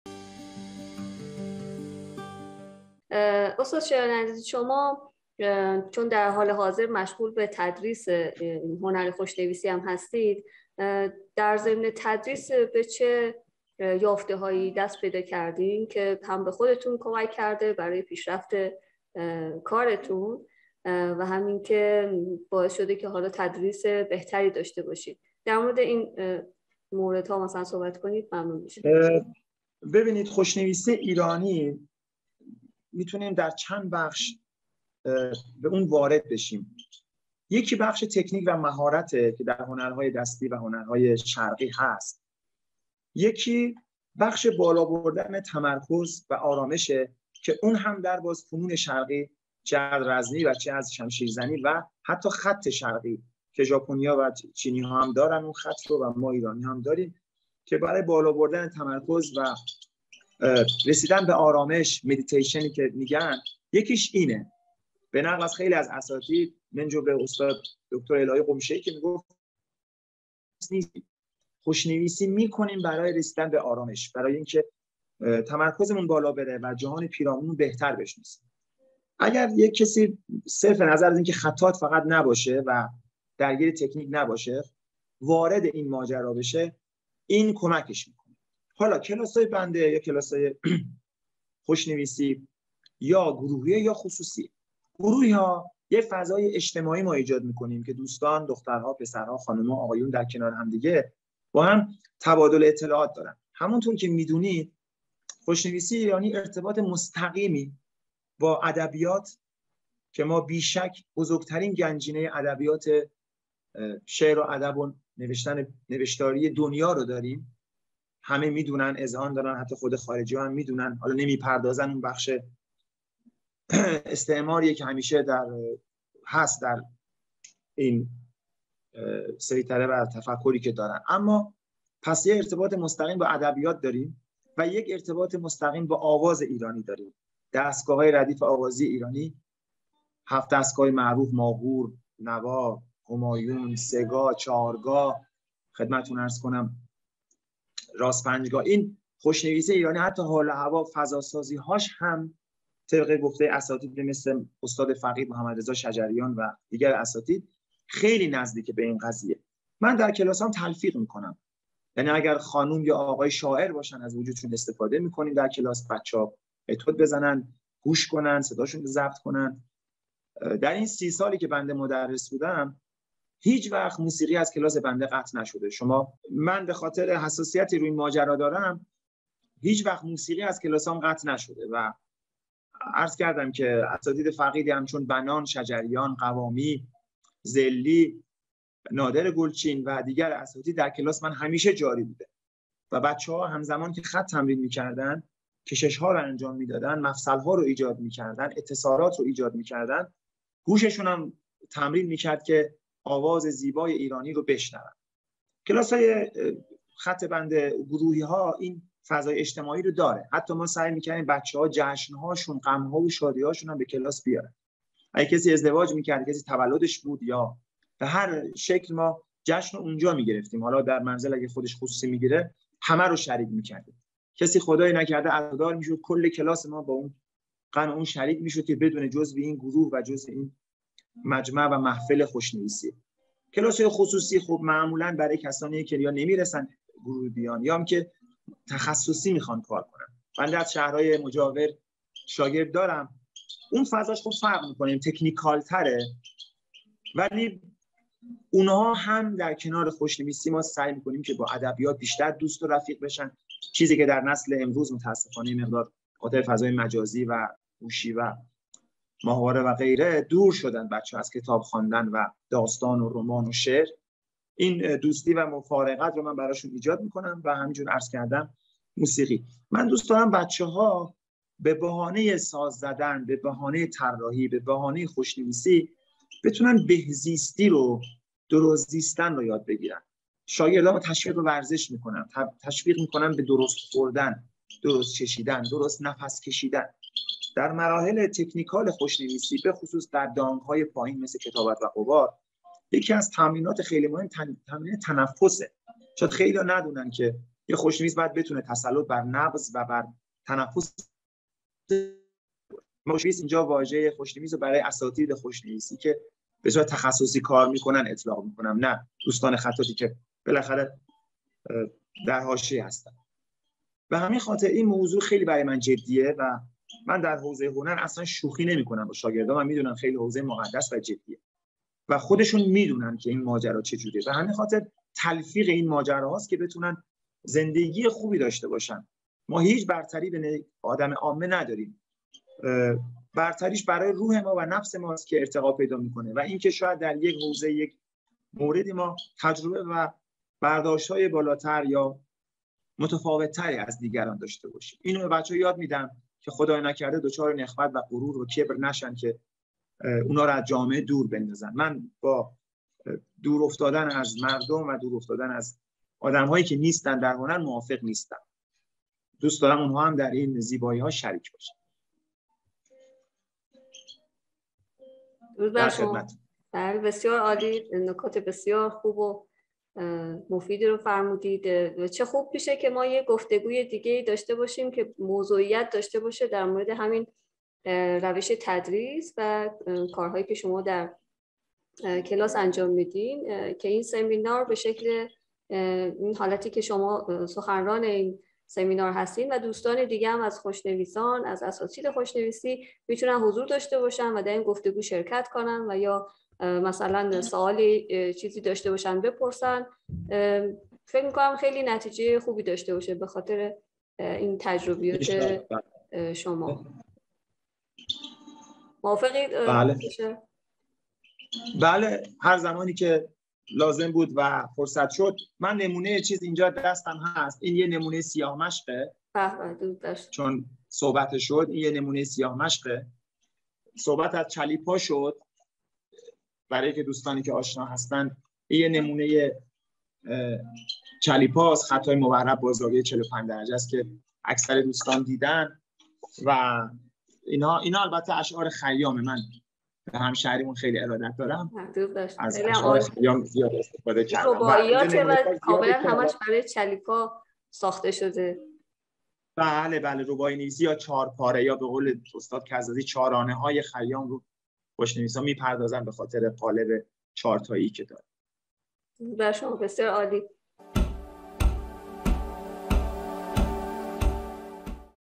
مروری بر مصاحبه با مجید شایان قسمت ششم
Majid Shayan Interview - Highlight Part 6

میهمان: مجید شایان
مجری: رویا دلخوش
موضوع: مدرس هنر و استاد خوشنويسي ایرانی
Guest: Majid Shayan
Host: Roya Delkhosh
Subject: Art teacher and master of Iranian calligraphy

آهنگساز:
حامد سلطانی
Composer:
Hamed Soltani